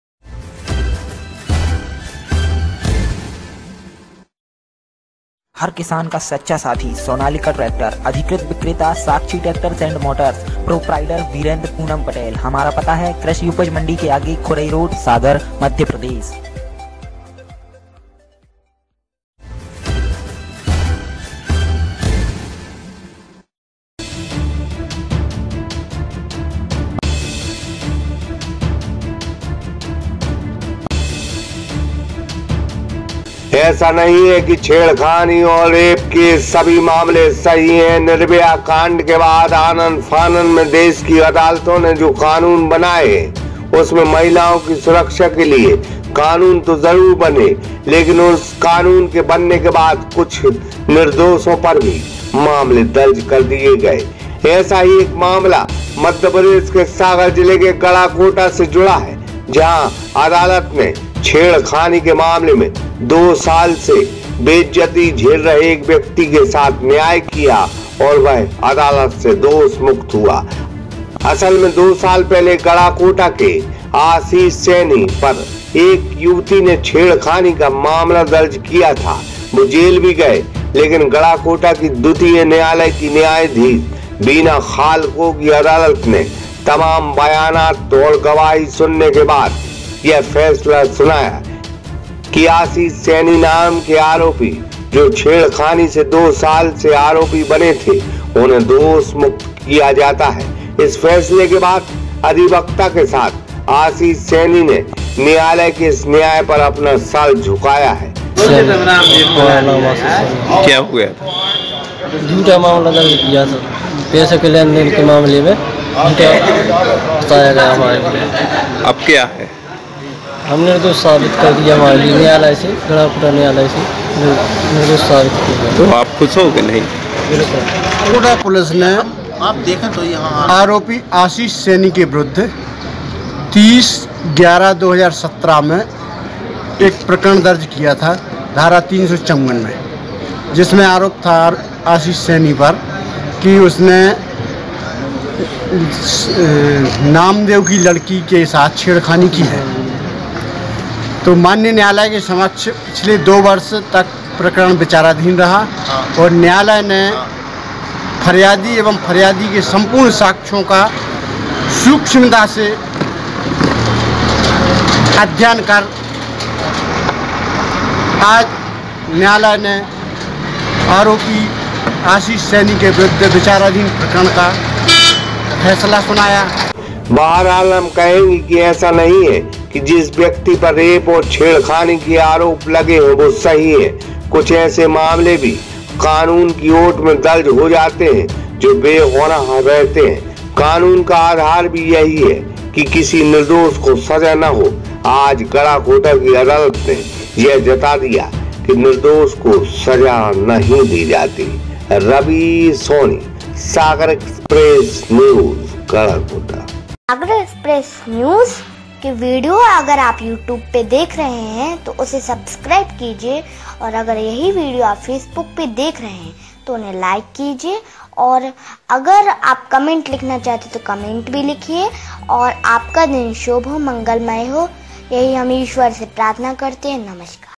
सभी छेड़खानी के मामले सही नहीं होते एक छेड़खानी का मामला झूटा हुआ
आदालत ने छेड़खानी को झूटा बताया